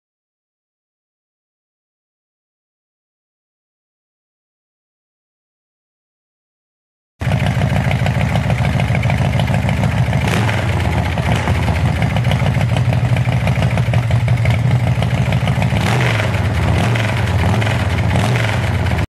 1999 Ultra Wide Drive One custom motorcycle for sale | sold at auction June 13, 2012
1999 Ultra Wide Drive One custom motorcycle
7,001 miles on odometer
S&amp;S 113 CID engine
1,852 cc
Five speed S&amp;S transmission
Belt driven
4.2 gallon dual fuel tanks
Arlen Ness 2" double chrome slasher exhaust
Custom flame paint job
Disk brakes
Custom billet wheels
90/90-21 front tire
180/55R18 rear tire
Owners manual
19ZWS31A5XC002053